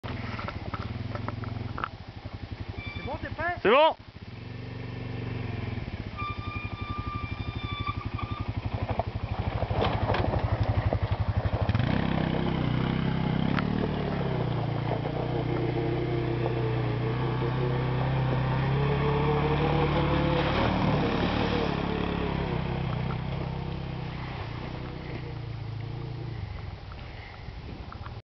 A mon tour de passer dans la boue
un Mxu 300 dans les bourbiers ca passe tout seul (enfin la c'est du bourbier de debutant mais faut bien commencer un jour)